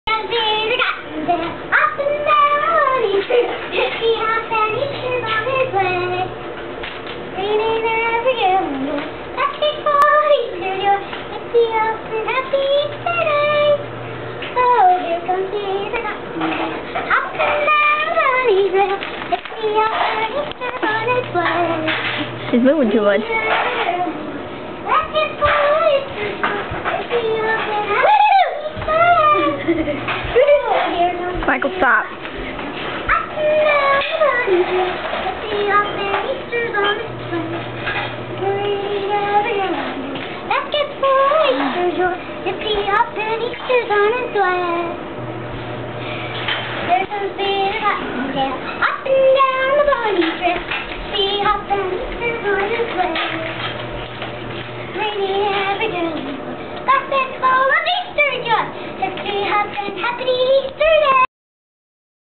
Little Sister's Easter Song
My little sister sings a song while setting Easter eggs, while her older brother tries to steal the show, and eventually takes the eggs and pelts her with them.